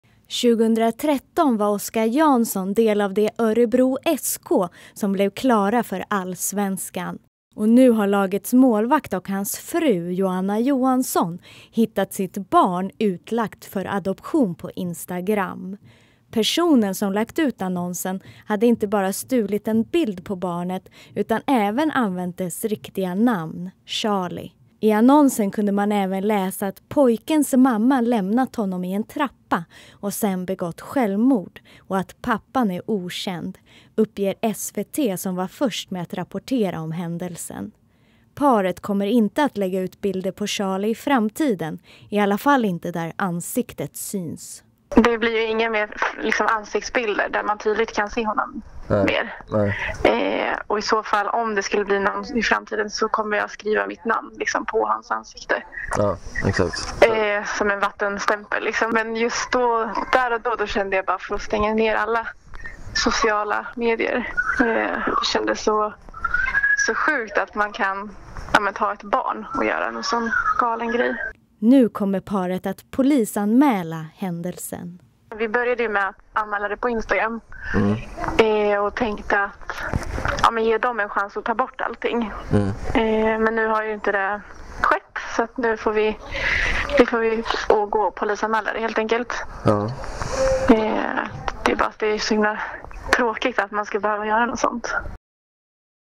Allsvenske Oscar Jansson hittade sin son utlagd för adoption på instagram
Paret polisanmäler händelsen.